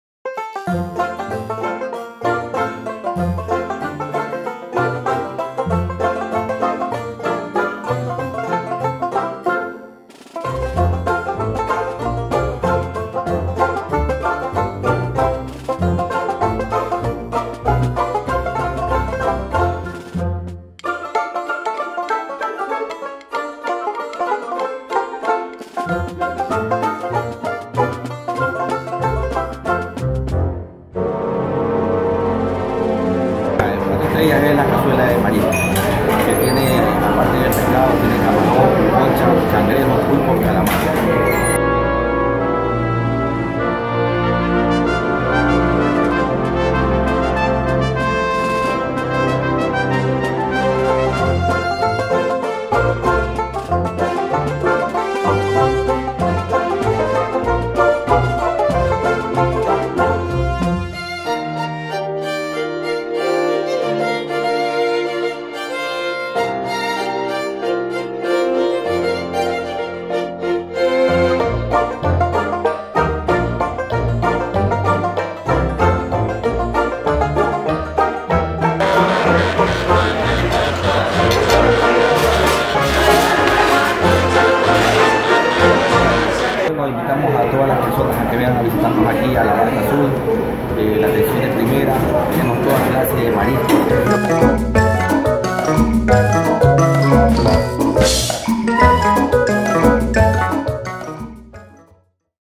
Huecas Guayaquil Cap 5- Cazuela
Con el equipo de Vértice, decidimos poder cumplir con las tradiciones guayaquileñas y eso es comer en las huecas reconocidas por todos y por el municipio de la ciudad.

En el quinto episodio nos fuimos a la hueca La Barca Azul a comer cazuela. Ubicada en Panamá 201 y Roca.